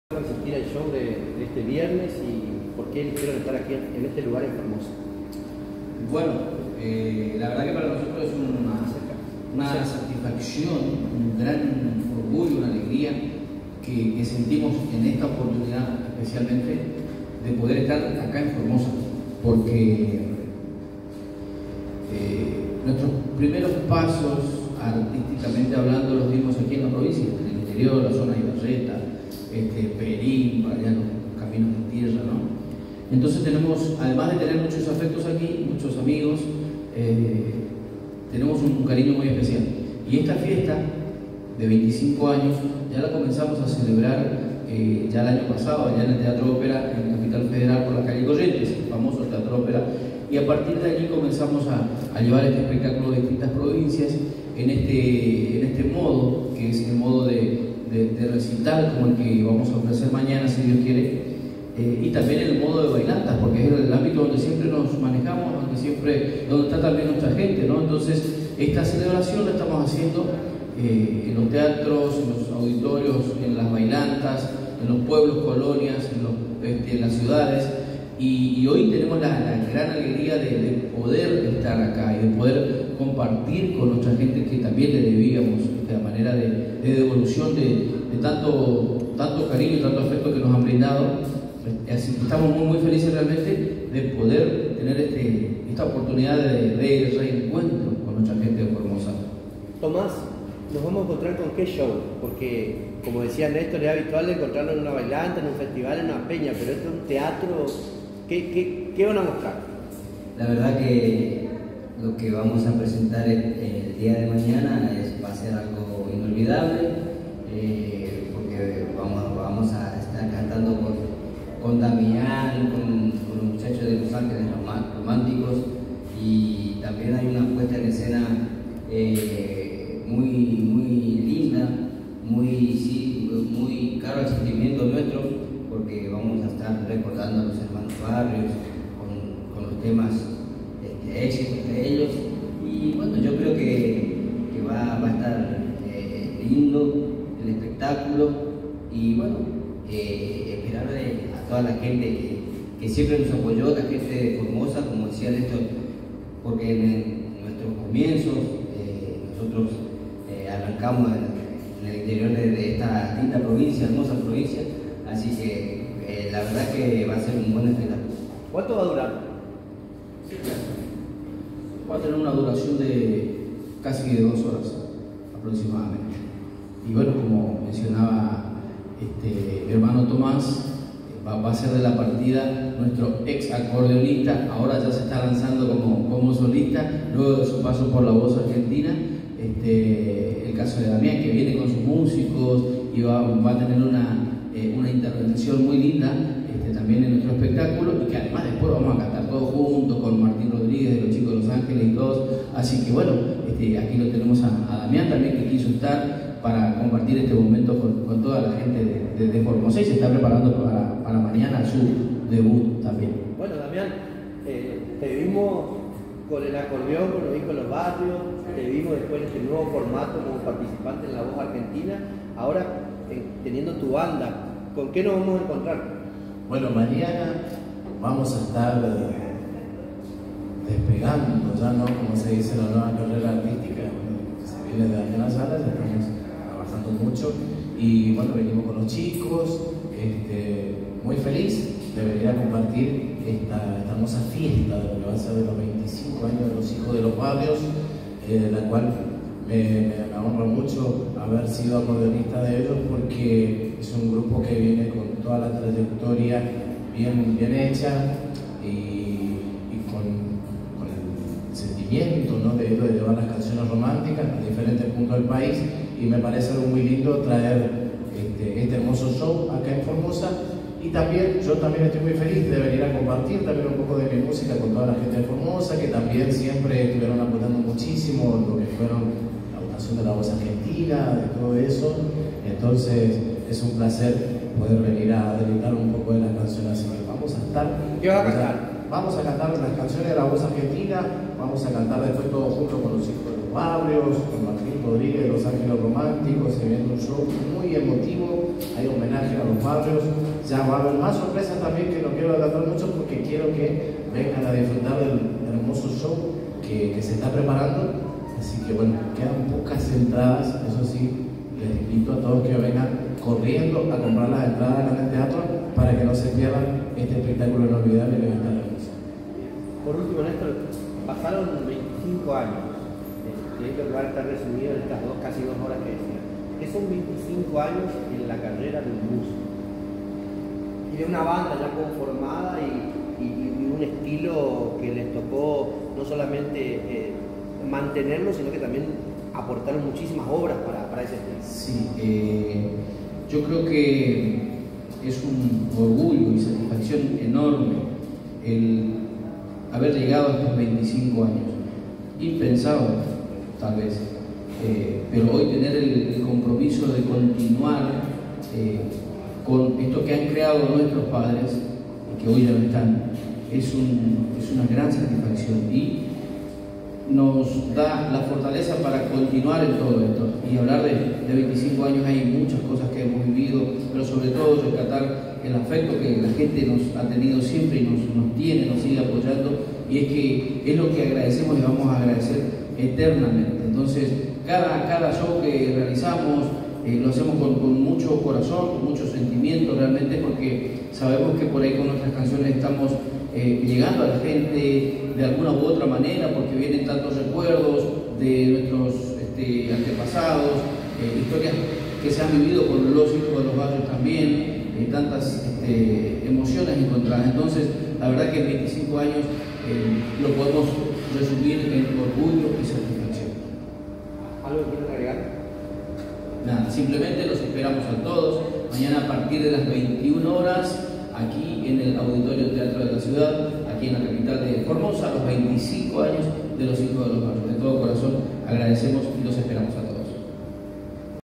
Conferencia de prensa de Los Hijos de los Barrios.
Conferencia de prensa de Los Hijos de los Barrios, promocionando el recital por los 25 años en el Auditorio Teatro de la Ciudad de Formosa.